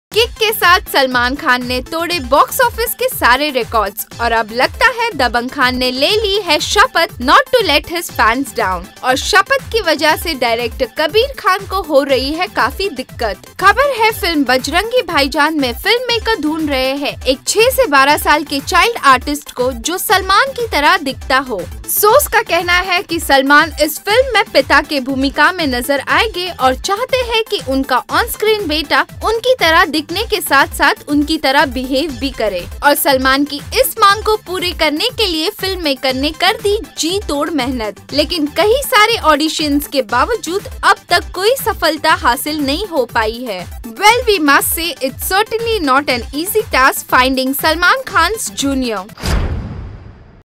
Finding Salman Khan's junior!
After giving a Kick ass performance at the box office, his next film, Bajraangi bhaijaan is giving the makers, sleepless nights. After loads of auditions Kabi Khan is yet to finalize a child artist to play Salman Khan's younger version.